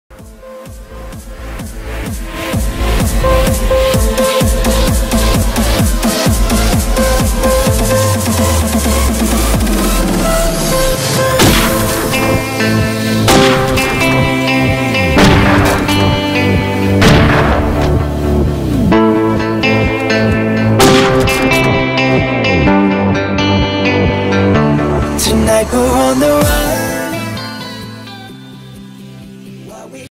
Flow. [OCC 38]
my entry for one clip contest week 38! i really went with the flow of the song for this video, with a unique beat that i hope you guys like. i hope its original enough to place this contest, give me all the support you can! :D all motivation keeps me going!